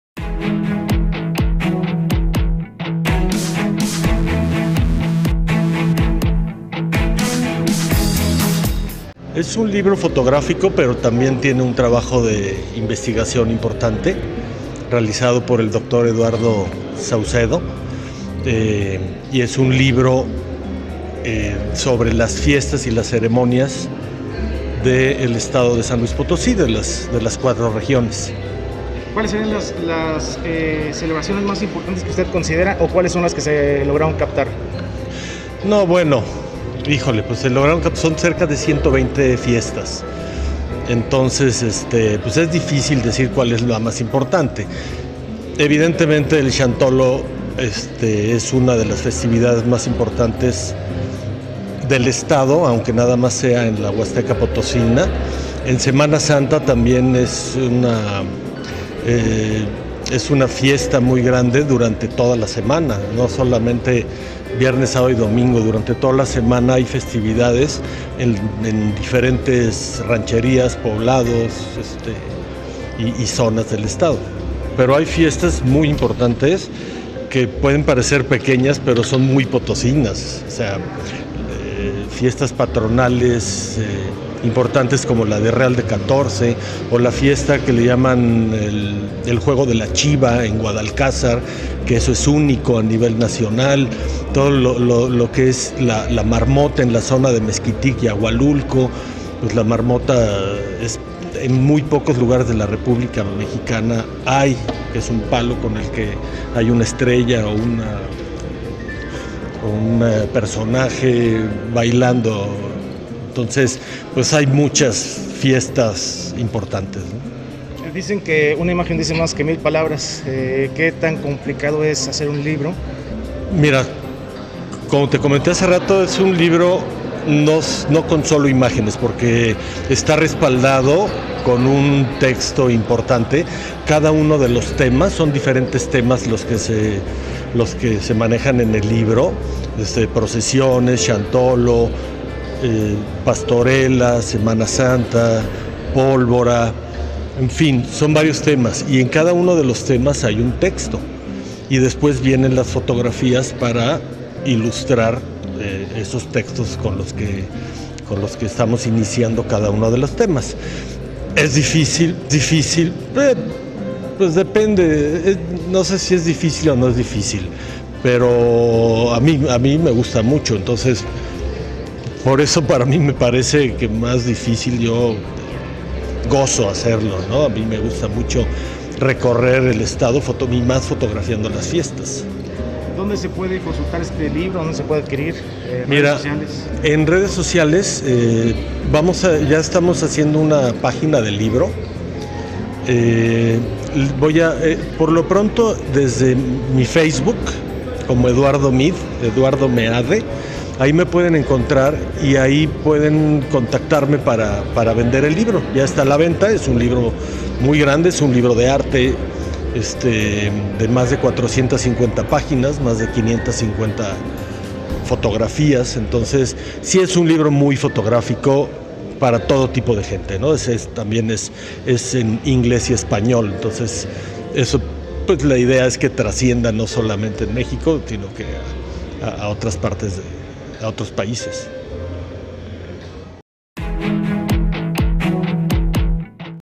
Presentación del libro Fiestas Ceremonias, legado de memoria y devoción - Eduardo Meade
El ayuntamiento de la capital de San Luis Potosí realizó la presentación del libro Fiestas Ceremonias, legado de memoria y devoción. Es un libro gráfico del fotógrafo potosino Eduardo Meade, dónde presenta la esencia social de las festividades más representativas del estado Potosino.